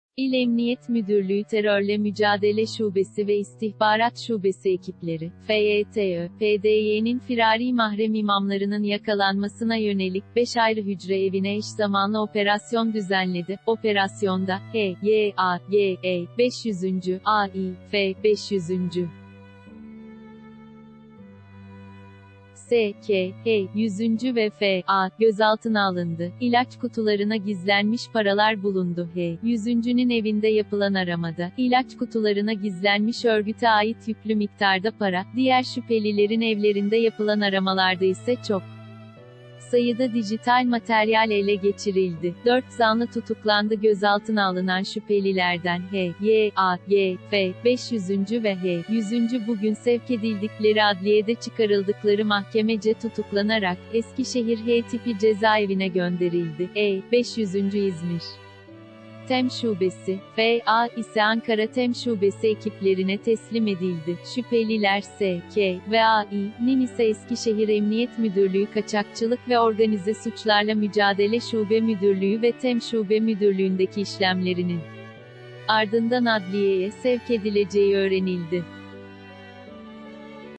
Eskişehir'de mahrem imam operasyonu: 4 tutuklama
Anita Andrews